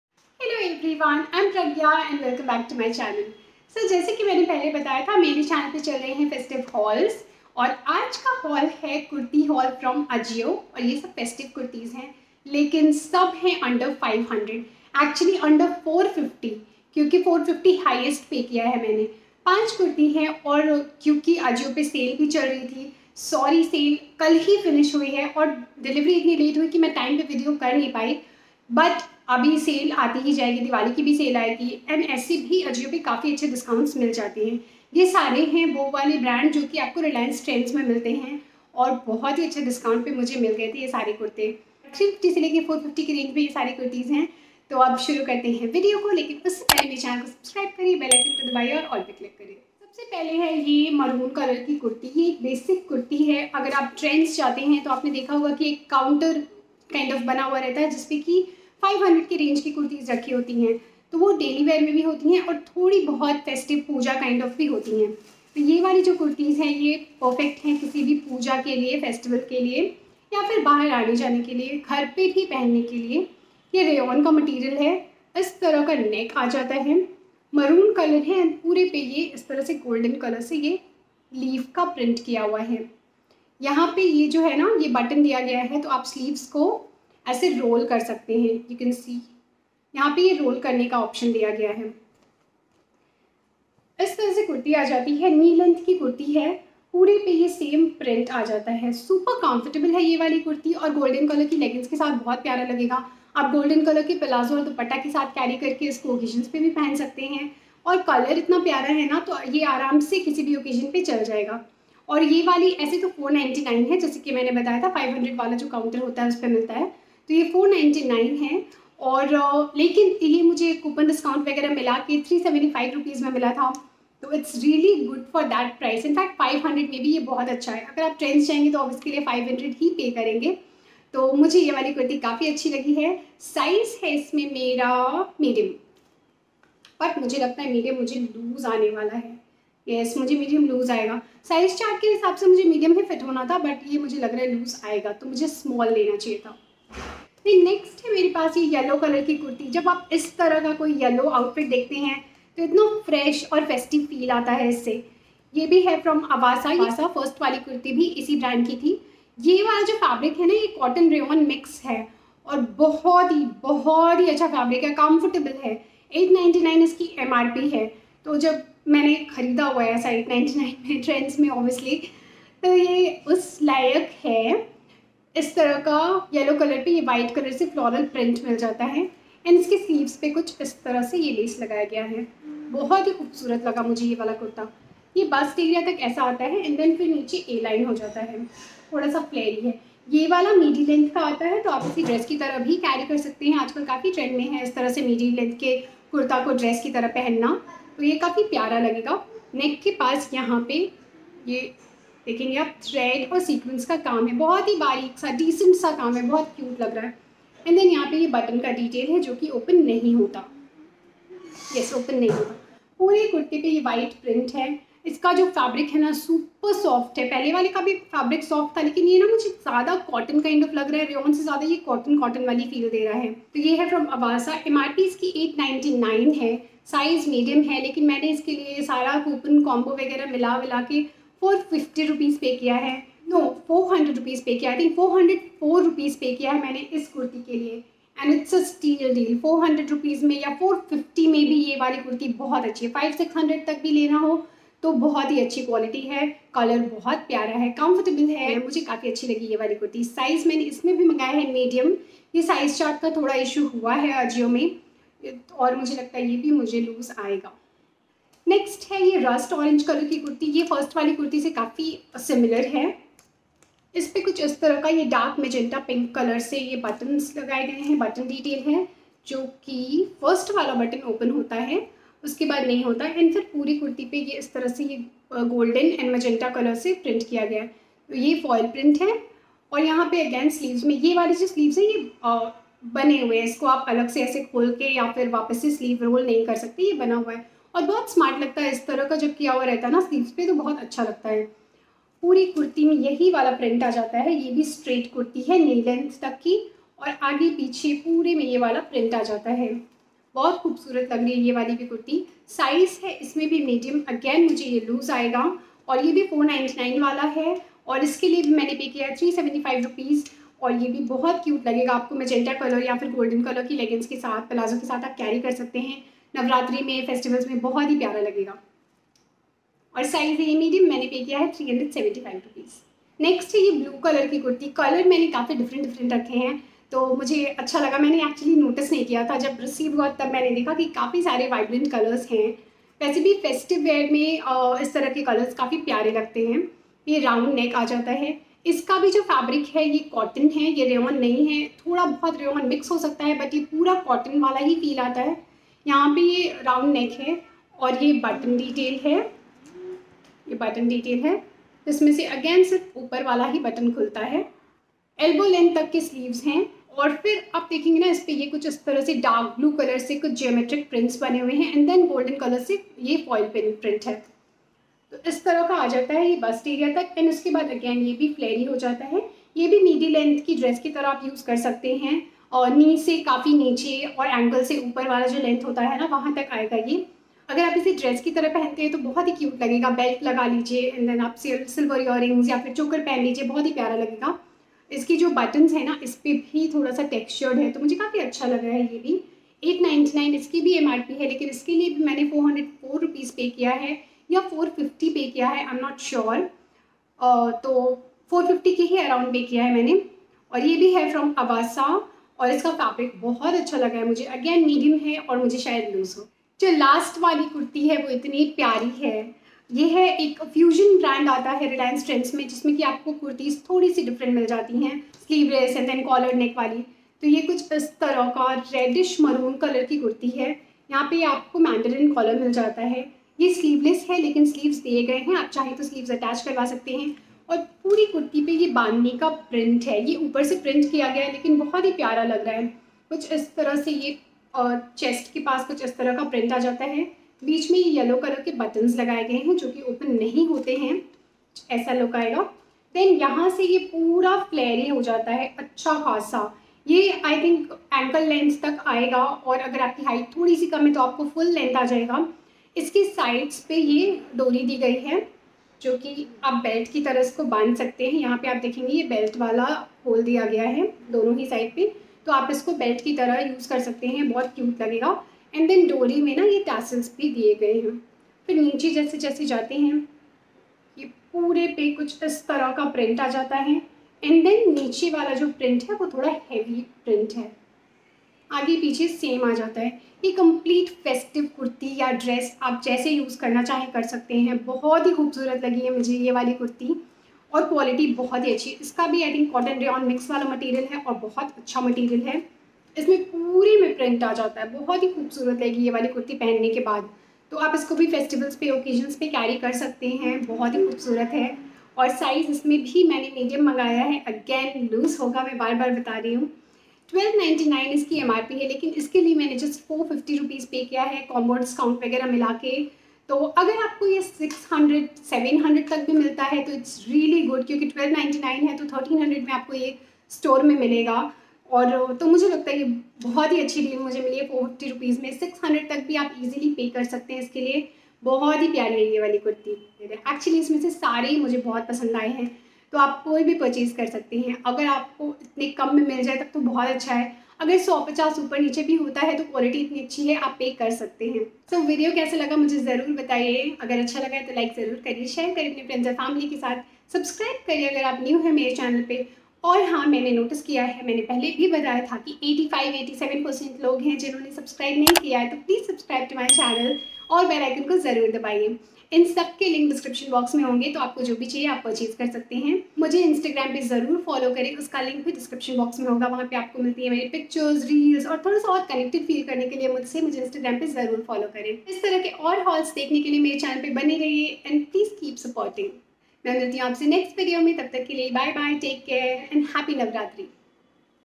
💛AJIO Big Bold Sale upto 90%off💛Festive kurti under 450💛Best Festive Kurta in budget💛ajiokurtahaul
Hi guys!!!
I have shared festive kurti under 450rs from AJIO in this video. I have purchased them from AJIO big bold sale and these are super affordable and of very good quality...I hope you all will like it ❤️

buying links

Thankyou for all your love and support💞